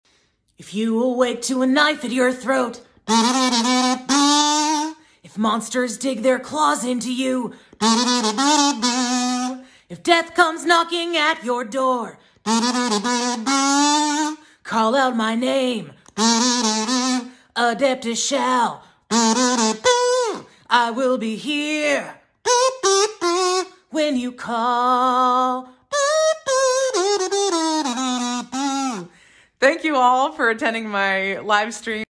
XIAO ENGLISH VOICE ACTOR AUTOGRAPHS SAT 2/13 - 2 PM PST/5 PM EST | GENSHIN IMPACT
Live streaming on Laila's TikTok - @lailaberzinsvoice

Hi! I'm Laila - The English Voice Actor for Xiao in Genshin Impact. Other notable roles include Milluki in HXH, Mrs. Croaker in Disney's Amphibia, Thorr in Fire Emblem Heroes, and Gondul the Valkryie in God of War 4.

Catch me this Saturday at 2pm PST/5 EST - I'll be signing Xiao Autographs live on my Youtube and Tiktok! Can't wait to see you! Yes, I'll bring the Kazoo!